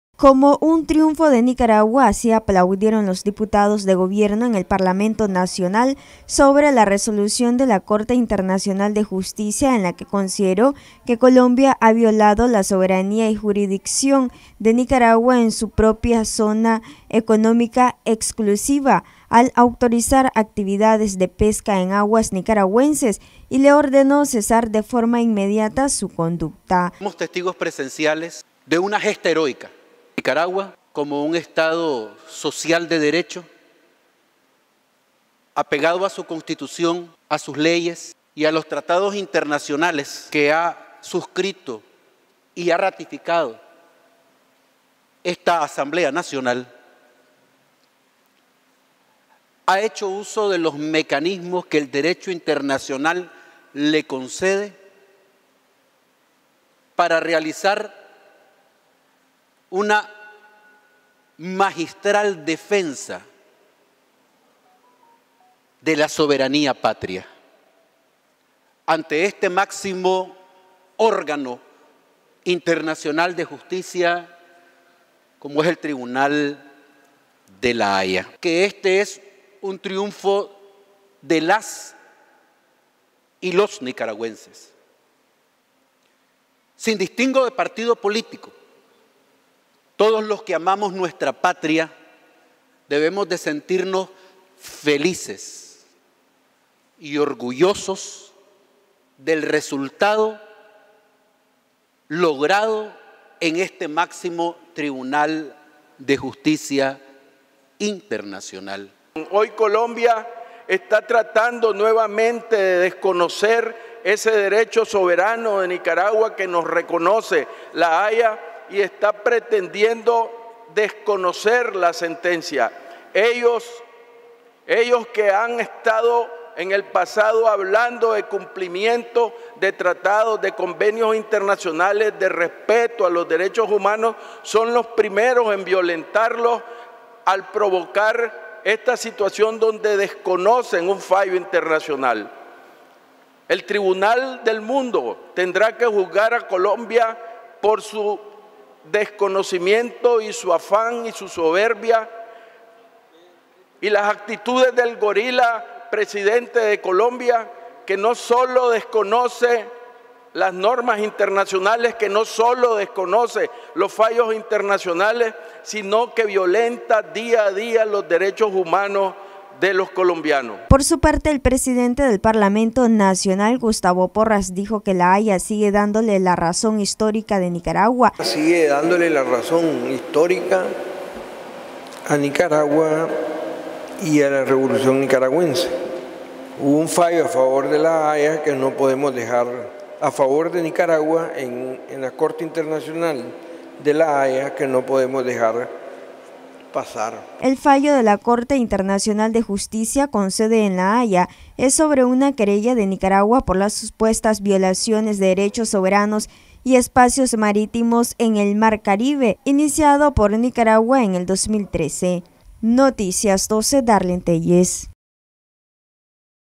Diputados de la bancada FSLN aplauden resolución de la Corte Internacional de Justicia
Noticias12Nicaragua Diputada del parlamento nacional, aplaudieron resolución de la Corte Internacional de Justicia de La Haya y afirman que es un triunfo para Nicaragua.